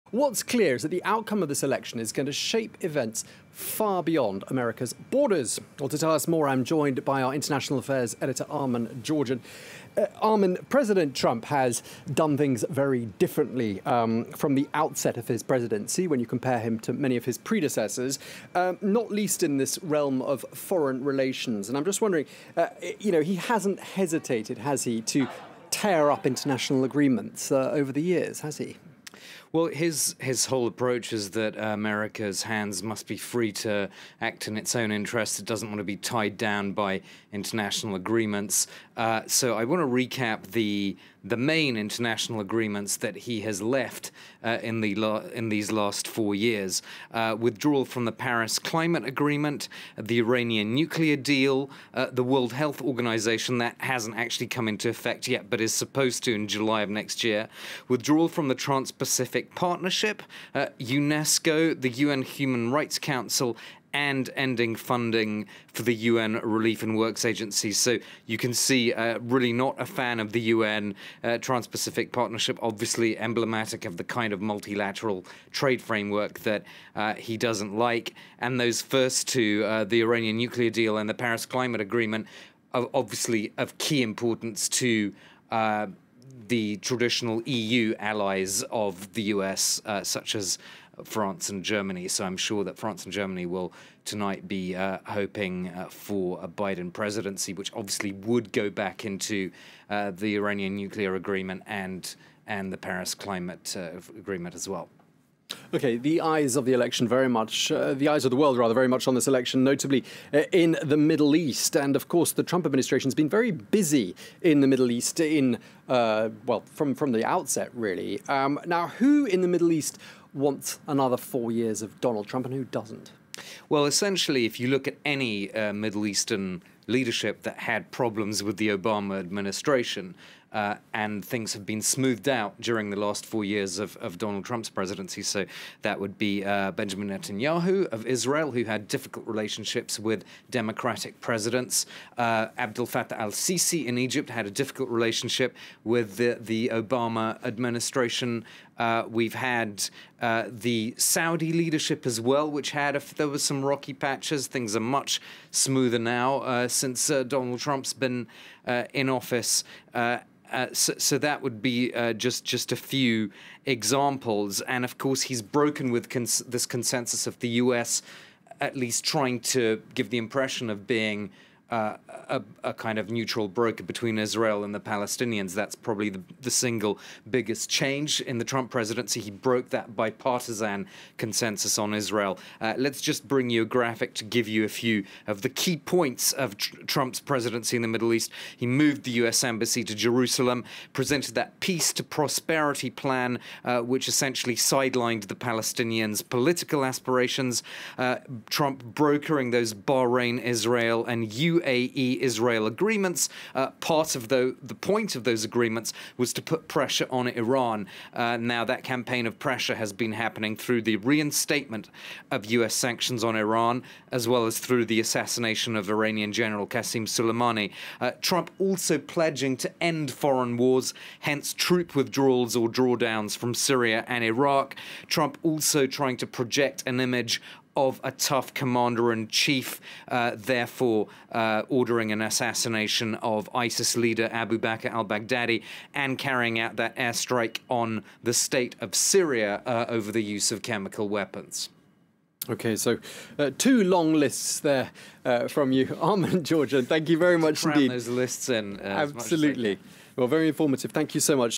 US election outcome will have consequences far beyond America's borders
What is clear is that the outcome of US Presidential election will have consequences far beyond America's borders. FRANCE 24's International Affairs editor Armen Georgian explains.